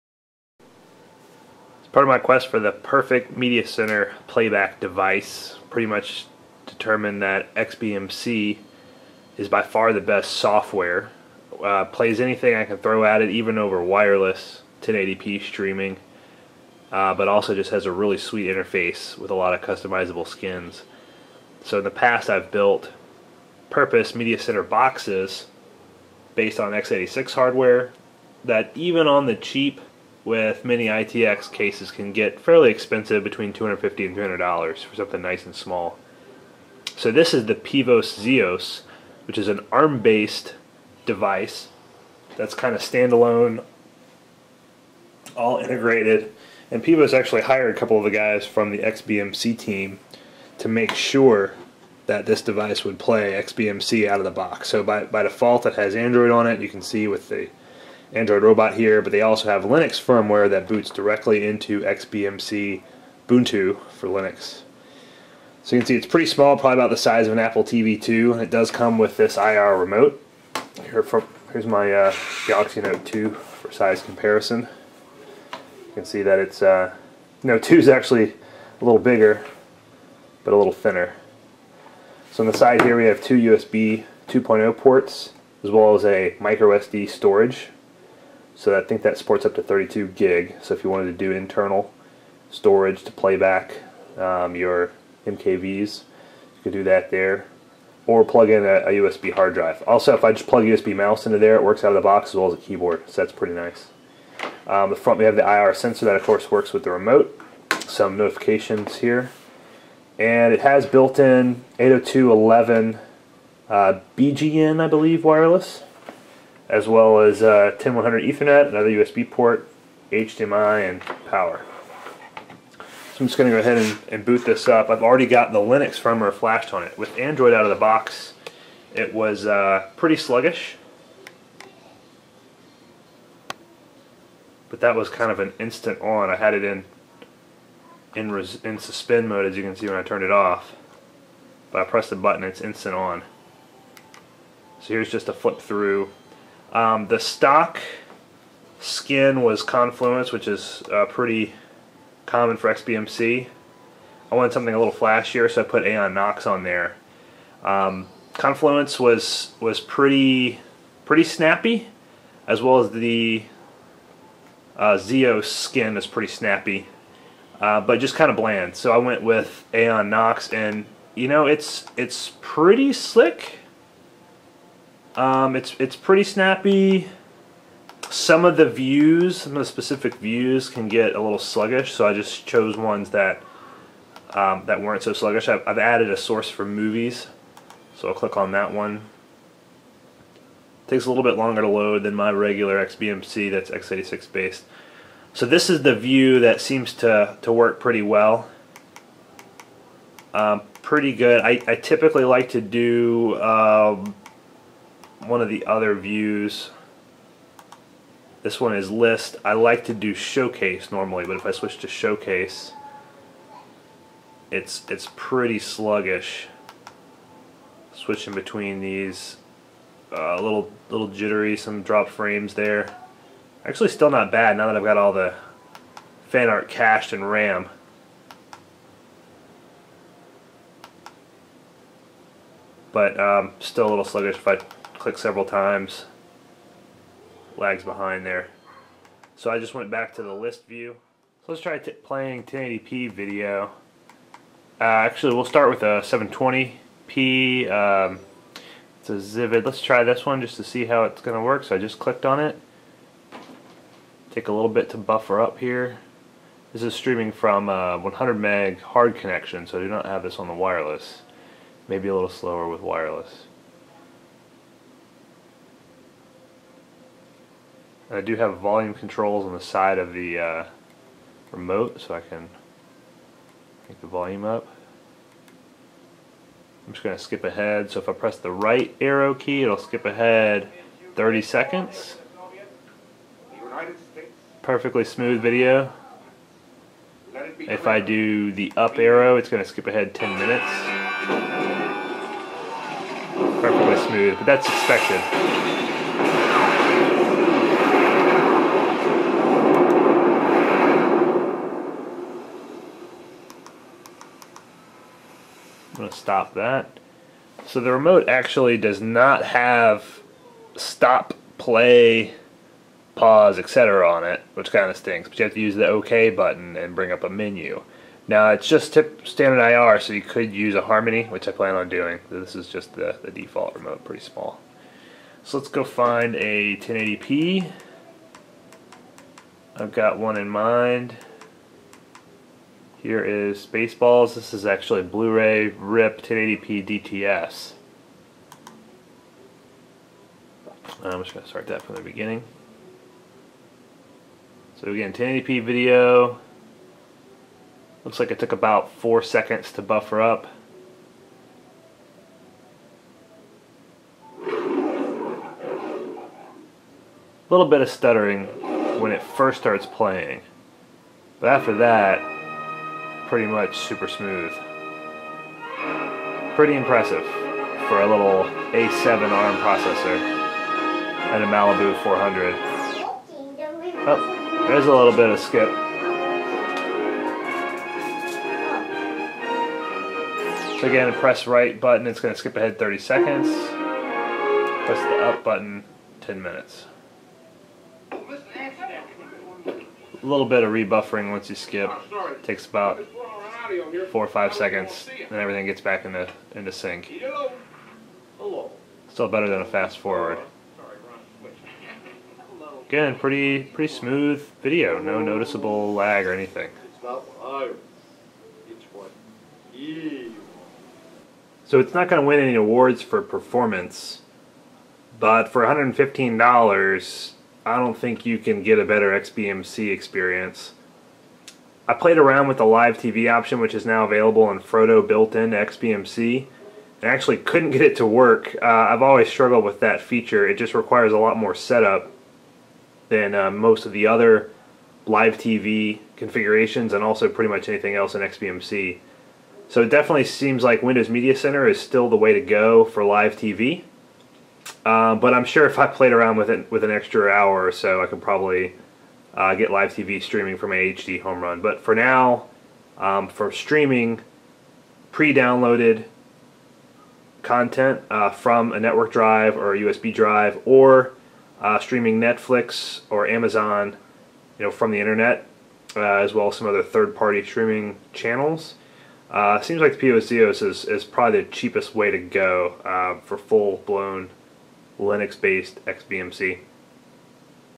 Demo - Pivos Xios XBMC-Linux
Pivos Xios M3 Flashed with the 1/23/2013 Linux Firmware.